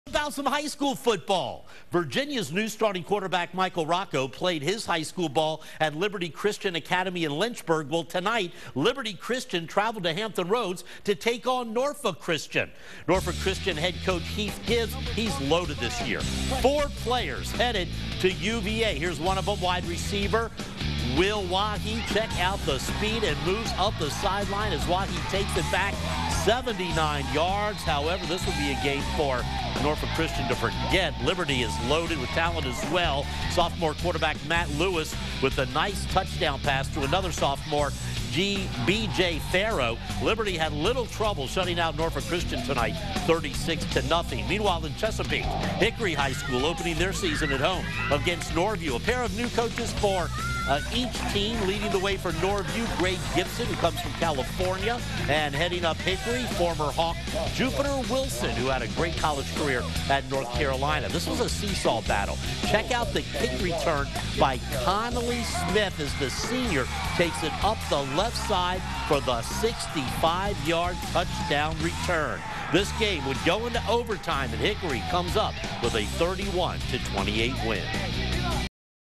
High School Football Kicks Off
Bruton 28, Greensville County 0

Catholic 28, Isle of Wight Academy 21

James Monroe 59, Tabb 20

Liberty Christian 36, Norfolk Christian 0

Hickory 31, Norview 28